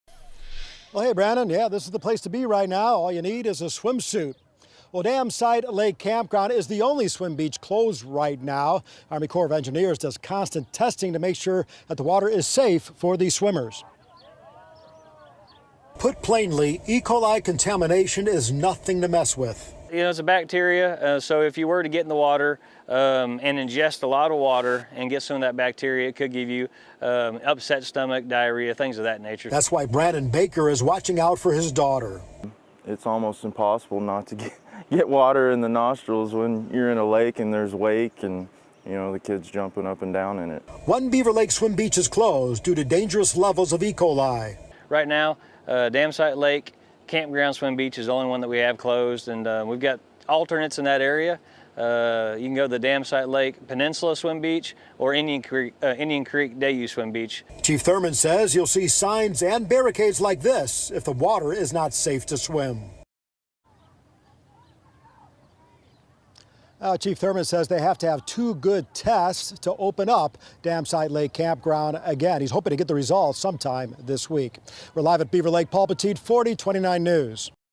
Confusion over swim beach closures at Beaver Lake
The best way to get the latest information on beach closures is to call the Beaver Lake Project office in Rogers at .